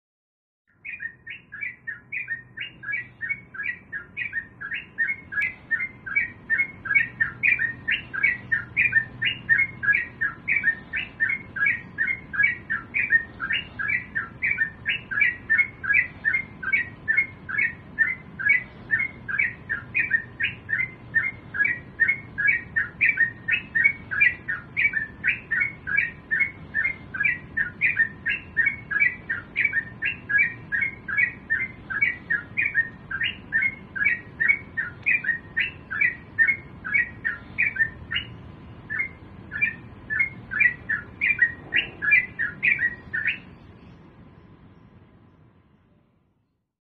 TRUCUKAN Gacor Di Pagi Hari
Suara Kicauan Burung Trucukan Di Pagi Hari Yg Cocok Untuk Di Jadikan Pancingan Untuk Trucukan Bahan Agar Cepat Bunyi